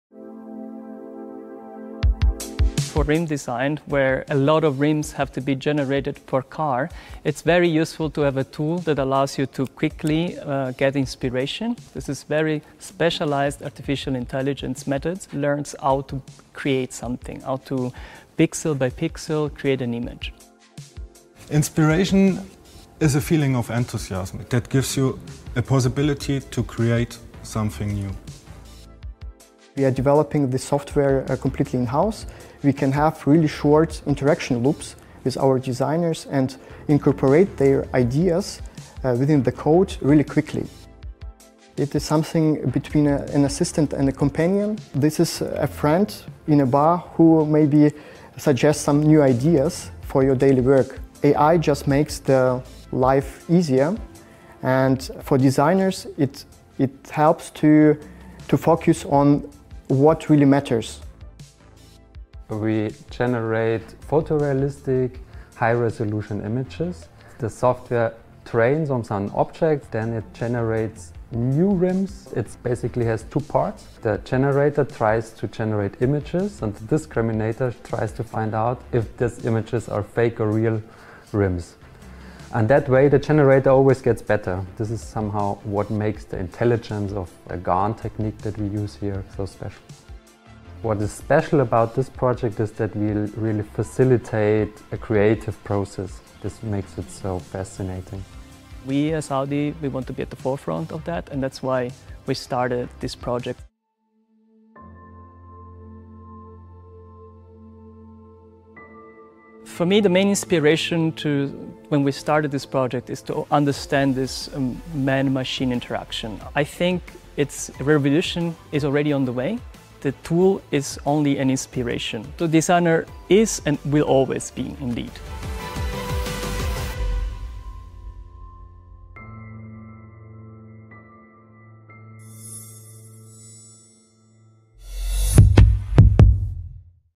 Using AI to Design Alloy Wheels
The design team at Audi has started creating alloy wheels using a new, in in-house developed artificial intelligence (AI) software called "FelGAN".